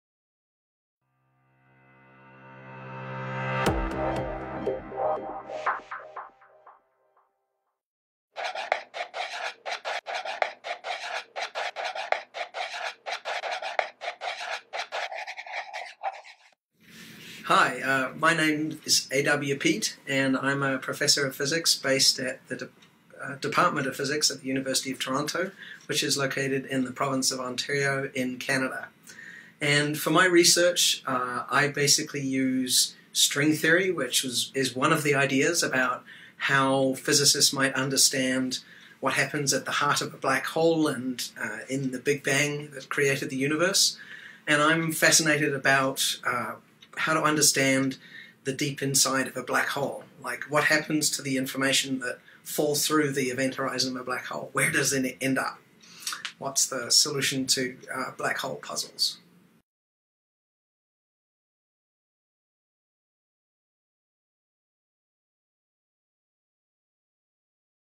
What I do as a Physicist | Professor A. W. Peet | University of Toronto
Professor A. W. Peet from the University of Toronto, Canada, describes in few words what it means to be a blackhole string theorist.